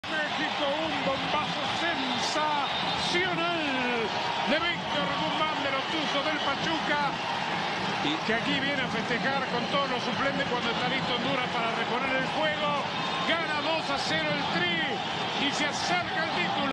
Gol del año: Guzmán vs Honduras.
Incondicionales, el 2015 fue un año de grandes goles y necesitamos su ayuda para decidir el mejor. ¿Será este "chanflazo" en la final del Preolímpico? ¡Participen!